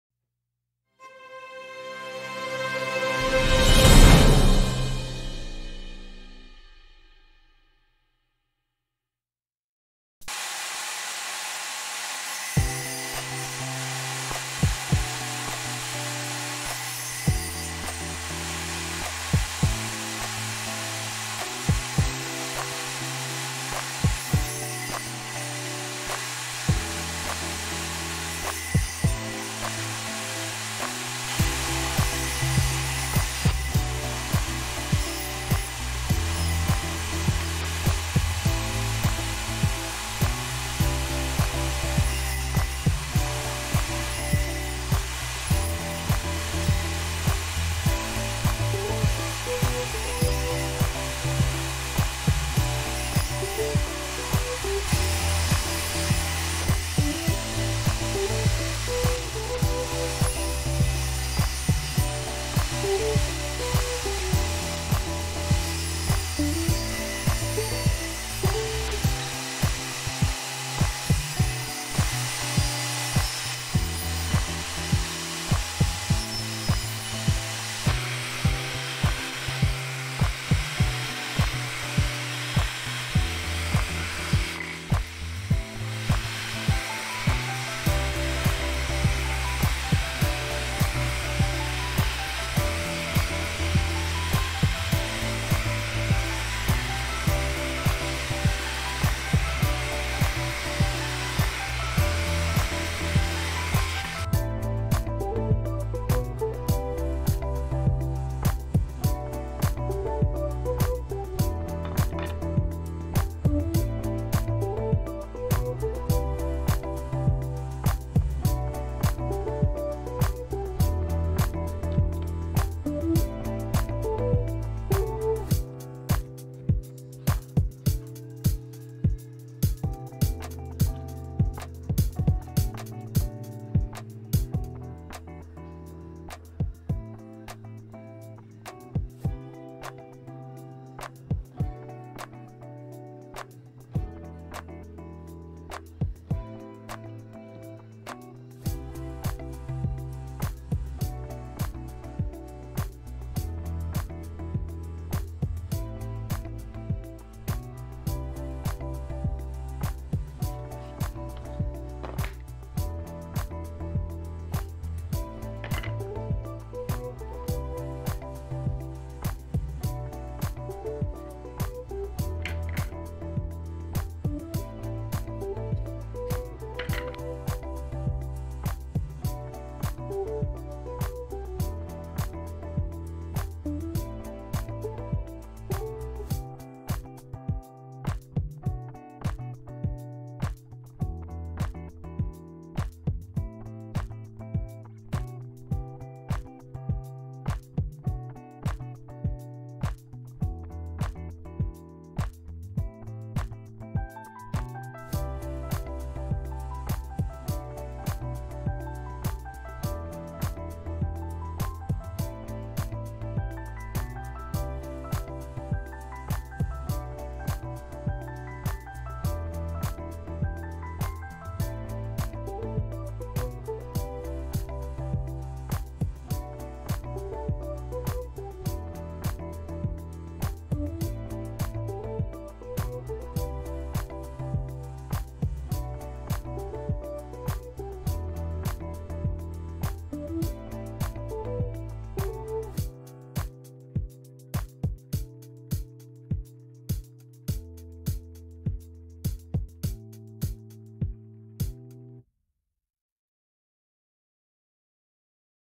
DIY- Smart Phone, Ipad Holder
A simple, easy to make smart phone and Ipad holder. Make your own! Enjoy watching.